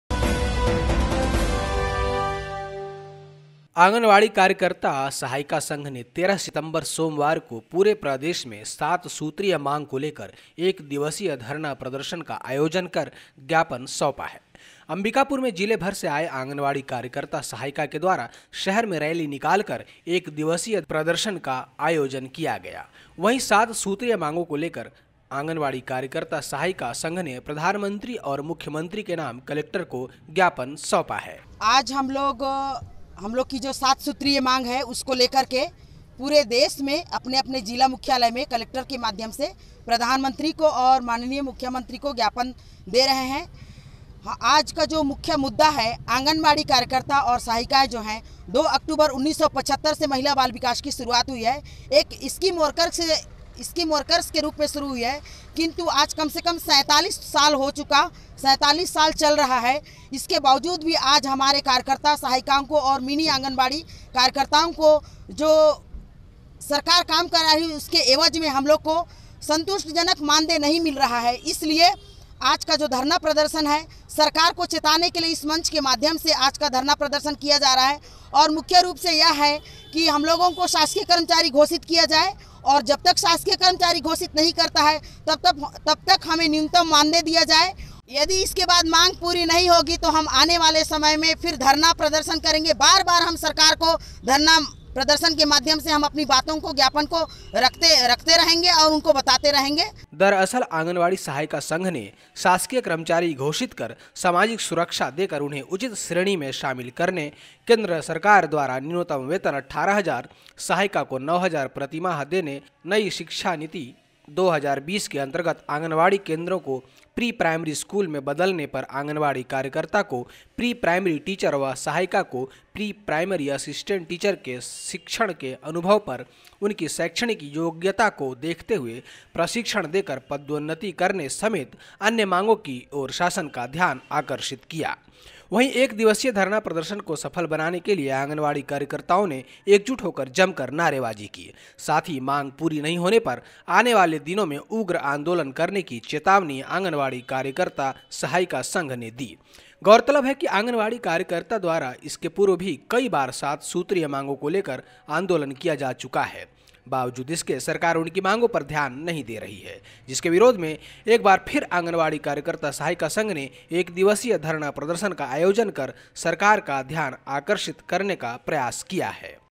आंगनबाड़ी सहायिका कार्यकर्ताओं ने किया प्रदर्शन...
आंगनबाड़ी कार्यकर्ता सहायिका संघ ने 13 सितंबर सोमवार को पूरे प्रदेश में 7 सूत्रीय मांग को लेकर एक दिवसीय धरना-प्रदर्शन का आयोजन कर ज्ञापन सौंपा है। अंबिकापुर में जिले भर से आए आंगनबाड़ी कार्यकर्ता सहायिका के द्वारा शहर में रैली निकालकर एक दिवसीय धरना प्रदर्शन का आयोजन किया गया, वही 7 सूत्रीय मांगों को लेकर आंगनबाड़ी कार्यकर्ता सहायिका संघ ने प्रधानमंत्री और मुख्यमंत्री के नाम कलेक्टर को ज्ञापन सौंपा है। दरअसल आंगनवाड़ी सहायिका संघ ने शासकीय कर्मचारी घोषित कर सामाजिक सुरक्षा देकर उन्हें उचित श्रेणी में शामिल करने, केंद्र सरकार द्वारा न्यूनतम वेतन 18000, सहायिका को 9000 प्रति माह देने, नई शिक्षा नीति 2020 के अंतर्गत आंगनबाड़ी केंद्रों को प्री-प्राइमरी स्कूल में बदलने पर आंगनबाड़ी कार्यकर्ता को प्री-प्राइमरी टीचर व सहायिका को प्री-प्राइमरी असिस्टेंट टीचर में शिक्षण के अनुभव पर उनकी शैक्षणिक योग्यता को देखते हुए प्रशिक्षण देकर पदोन्नत करने समेत अन्य मांगों की ओर शासन का ध्यान आकर्षित किया गया। वही एक दिवसीय धरना प्रदर्शन को सफल बनाने के लिए आंगनबाड़ी कार्यकर्ताओं ने एकजुट होकर जमकर नारेबाजी।
साथ ही मांग पूरी नहीं होने पर आने वाले दिनों में उग्र आंदोलन करने की चेतावनी आंगनवाड़ी कार्यकर्ता सहायिका संघ ने दी है। गौरतलब है कि आंगनबाड़ी कार्यकर्ताओं द्वारा इसके पूर्व भी कई बार 7 सूत्रीय मांगों को लेकर आंदोलन किया जा चुका है। बावजूद इसके सरकार उनकी मांगों पर ध्यान नहीं दे रही है। जिसके विरोध में एक बार फिर आंगनबाड़ी कार्यकर्ता सहायिका संघ ने एक दिवसीय धरना प्रदर्शन का आयोजन कर सरकार का ध्यान आकर्षित करने का प्रयास किया है।